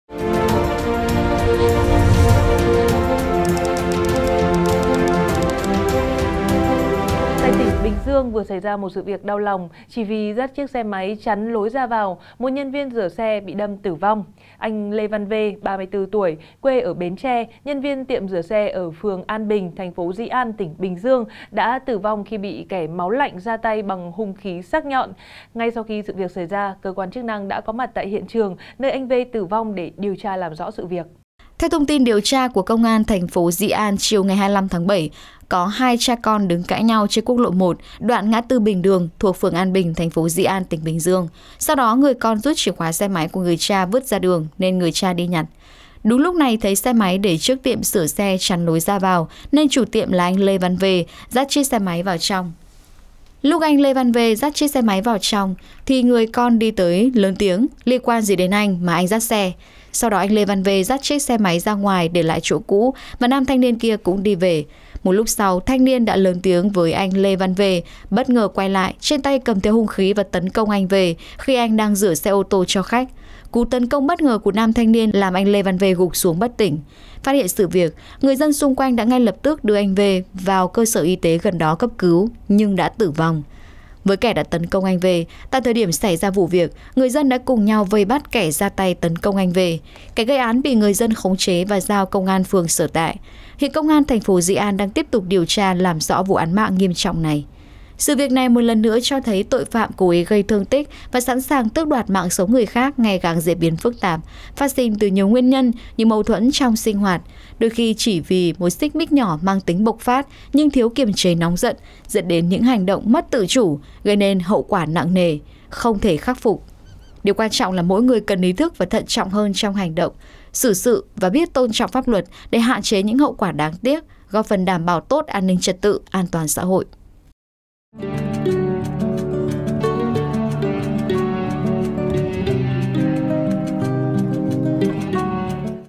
Hé Lộ Nguyên Nhân Nhân Viên Rửa Xe Bị Sát Hại Tại Bình Dương | SKĐS
SKĐS | Anh V. (34 tuổi), nhân viên tiệm rửa xe tại TP Dĩ An, tỉnh Bình Dương đã bị một người đàn ông ra tay bất ngờ bằng hung khí sắc nhọn. Anh V. tử vong sau đó, kẻ gây án cũng đã bị bắt giữ. Vậy nguyên nhân gì dẫn đến vụ sát hại đau lòng trên?

Mời quý vị và các bạn xem thêm: Nhân Chứng Bàng Hoàng Kể Lại Phút Đập Cửa Vụ 6 Người Trong Một Nhà Tử Vong Ở Bình Dương